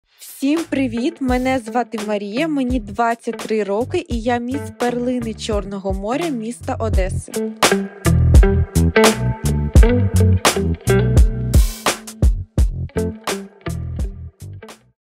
Учасниця «Міс Україна 2024» №6 Желяскова Марія, Одеська область
Учасниця №6 Желяскова Марія, Одеська область.

Володарка титулу «Міс Одеса 2024».
За фахом — диктор і телеведуча, що стало основою модельної кар’єри.

Серце Марії належить благодійності: допомагає дитячим будинкам, притулкам для тварин, підтримує військових та дітей, які постраждали від війни. Її мета — захищати права жінок і допомагати тим, хто бореться за нашу свободу.